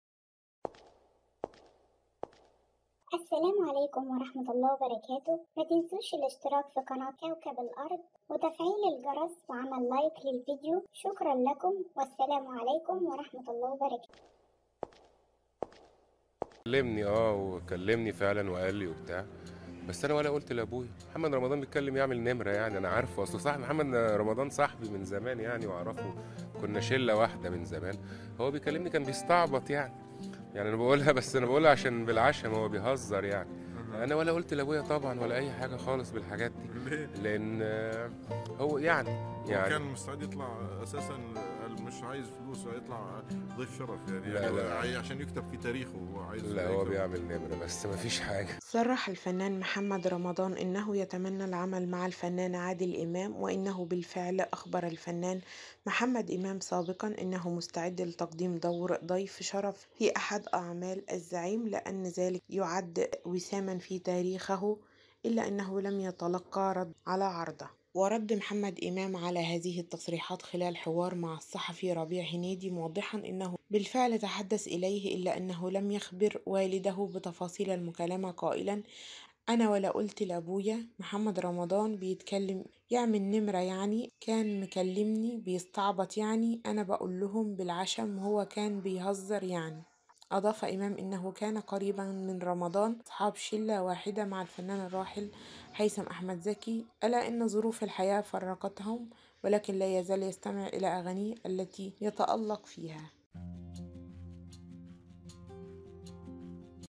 بالفيديو- محمد إمام يرد على رمضان: بيستعبط ويعمل نمرة
صرح الفنان محمد رمضان أنه يتمنى العمل مع الفنان عادل إمام وأنه بالفعل أخبر الفنان محمد إمام سابقًا أنه مستعد لتقديم دور ضيف ‏شرف في أحد أعمال الزعيم لأن ذلك يعد وسامًا في تاريخه، إلا أنه لم يتلق ردًا على عرضه.‏